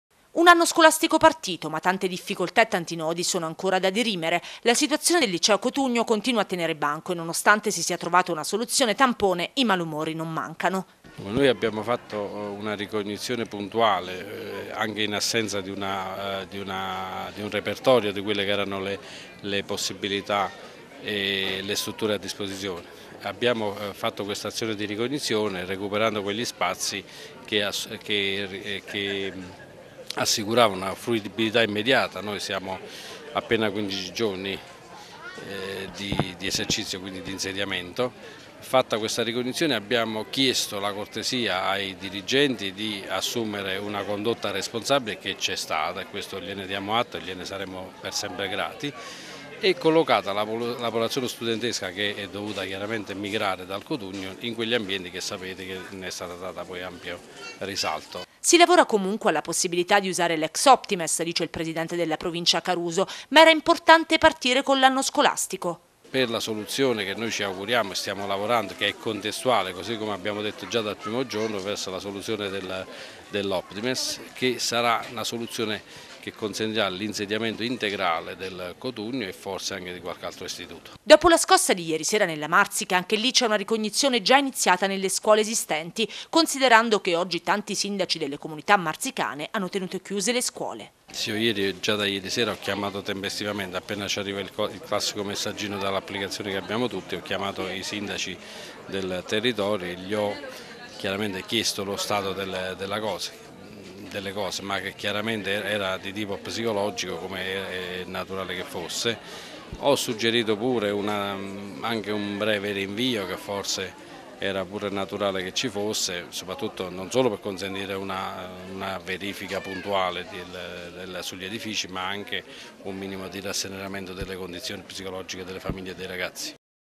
L’Aquila. Il punto sul nuovo anno scolastico
Un inizio anno scolastico a L'Aquila caratterizzato da incertezze e malumori. La soluzione trovata per il Liceo - assicura il presidente della Provincia - non è definitiva, lavoriamo su una altra struttura ma l'anno scolastico doveva partire e abbiamo ascoltato le istanze di tutti per iniziare in sicurezza.